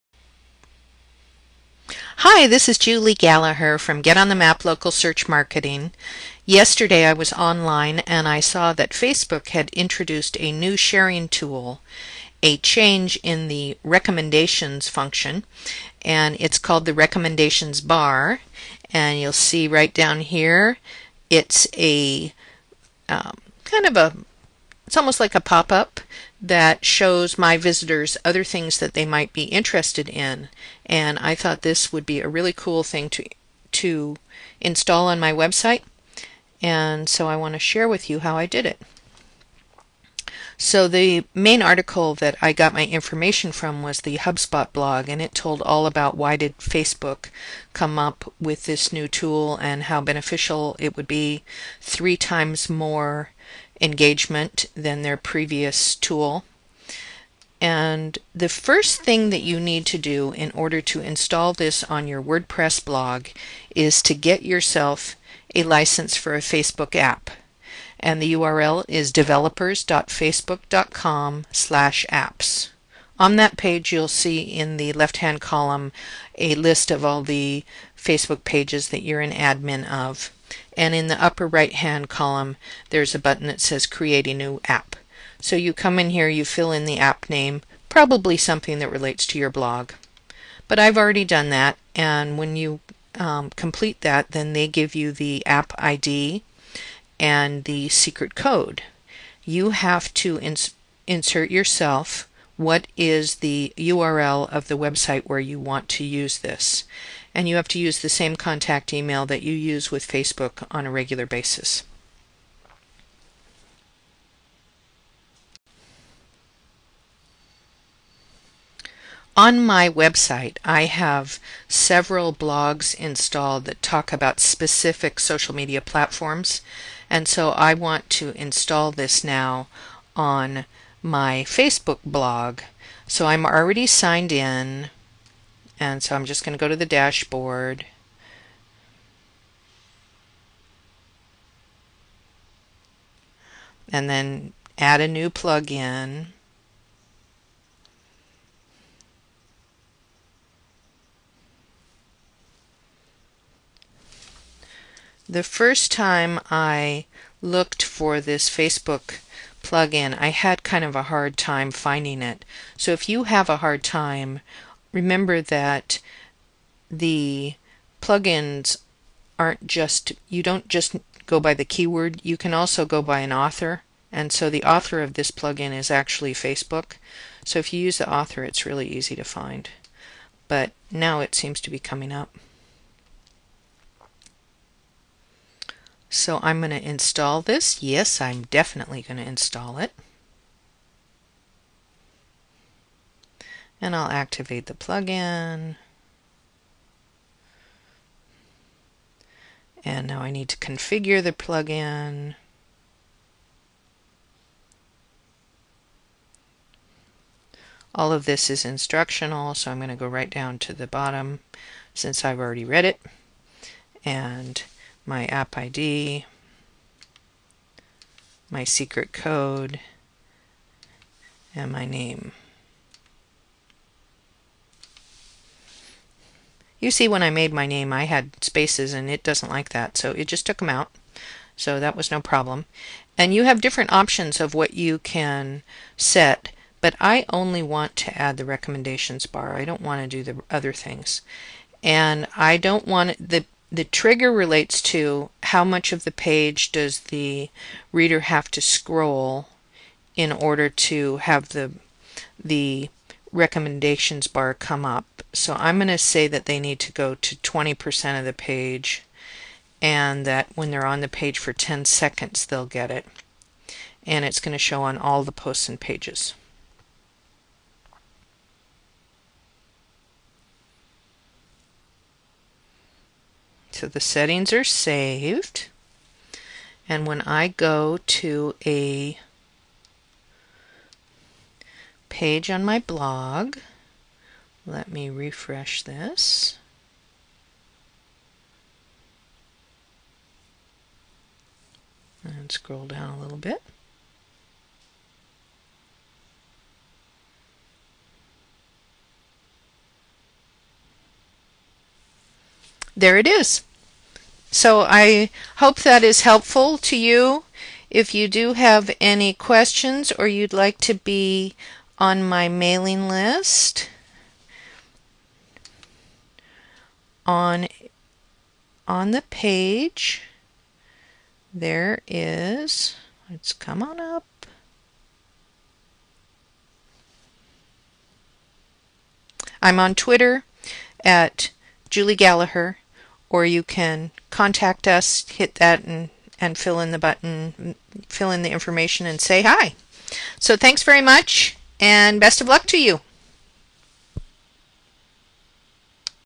How to Install Facebook Recommendations Bar on Your Wordpress Blog
demonstrates installation of Facebook Wordpress plug-in to install new Facebook Recommendations bar tool.